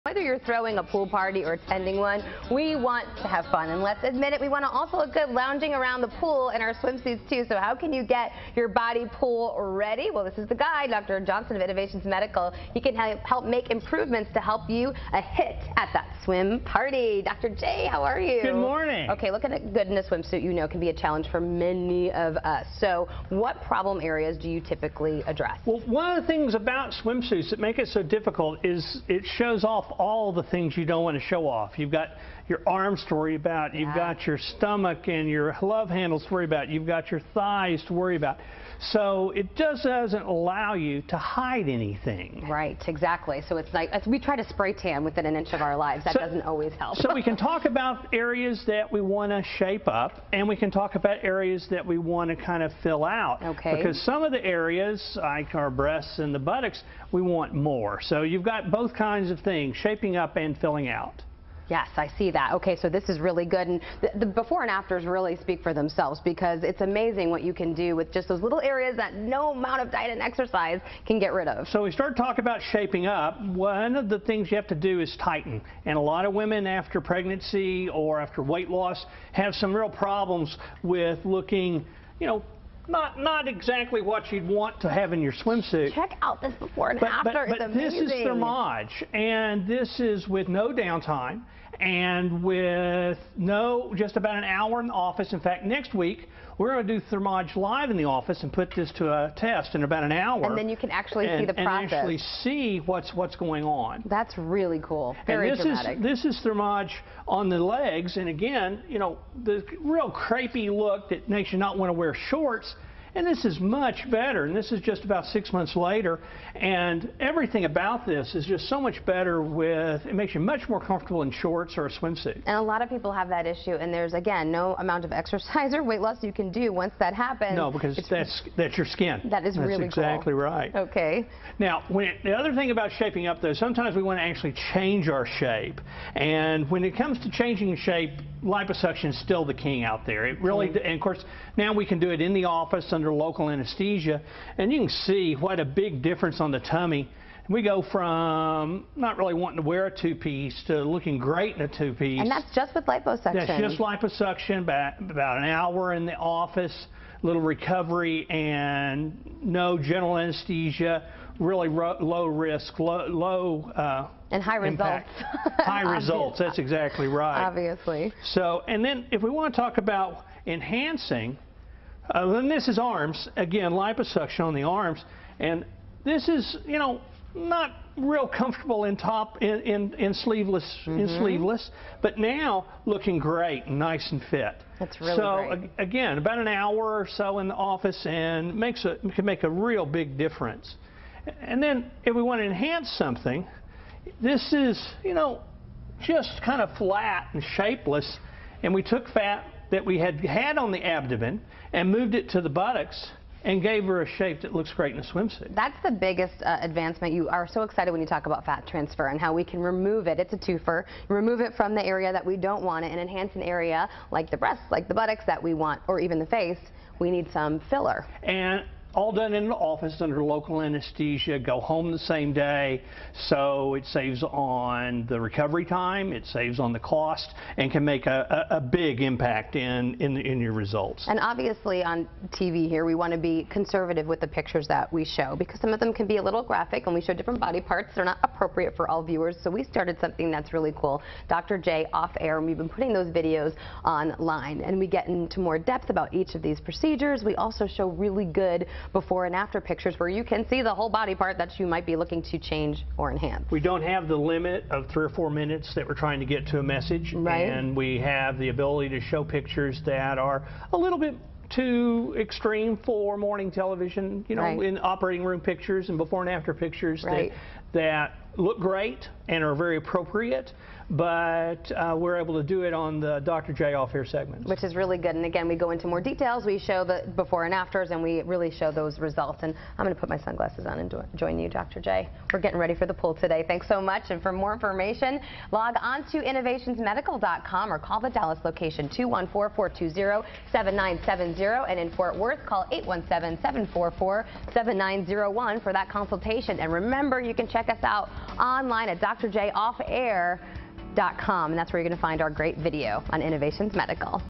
Get your body pool party ready! It's not too late to look great in 2014
Host: Whether you’re throwing a pool party or ending one. We want to have fun and let’s admit it we want to also a good lounging around the pool in our swimsuit too. So how can you get your body pull already. Well this is the guy Dr. Johnson of Innovations Medical you can help make improvements to help you a hit at that swim party. Dr. J how are you?

Dr. J: Good morning.

Host: Okay look at goodness swim suit you know can be a challenge for many of us so what problem areas do you typically address.

Dr. J: Well one of the things about swimsuits that make it so difficult is it shows off all the things you don’t want to show off you’ve got your arm story about you’ve got your stomach and your love handles. Worry about you’ve got your thighs to worry about, so it doesn’t allow you to hide anything.

Host: Right exactly so it’s like we try to spray tan within an inch of our lives doesn’t always help.

Dr. J: So, we can talk about areas that we want to kind of fill out okay because some of the areas like are breast and the buttocks we want more so you’ve got both kinds of things shaping up and filling out.

Host: Yes, I see that okay, so this is really good and the before and after really speaks for themselves because it’s amazing. What you can do with just those little areas that no amount of diet and exercise can get rid of.

Dr. J: So, we start talking about shaping up one of the things you have to do is tighten and a lot of women after pregnancy or after weight loss have some real problems with looking you know not exactly what you’d want to have your swimsuit. This is the lodge, and this is with no downtime and with just about an hour an office in fact next week we’re going to do thermage live in the office and put this to a test in about an hour and you can actually see what’s going on. This is thermage on the legs and again you know the real creepy look that makes you not want to wear shorts and this is much better, and this is just about six months later and everything about this is just so much better with it makes you much more comfortable in shorts or a swimsuit.

Host: A lot of people have that issue and there’s again no amount of exercises weight less. You can do once that happens.

Dr. J: No because that’s your skin. Now when the other thing about shaping up there sometimes we want to actually change our shape and when it comes to changing shape liposuction still the king out there it really and of course now we can do it in the office under local anesthesia and you can see what a big difference on the tummy. We go from not really wanting to wear a two piece to looking great in a two piece. That’s just with liposuction back about an hour in the office little recovery and no general anesthesia really low risk.

Host: And hiring back in results.

Dr. J: That’s exactly and so if we talk about enhancing then this is arms again liposuction on the arms and this is you know not real comfortable in top, in sleeveless but now looking great nice and fit. Let’s so again about an hour or so in the office and makes it could make a real big difference.

LOSE AN INCH IN ONE HOUR

IS YOUR BODY SUMMER READY?

Innovations Medical
Innovations Stem Cell Center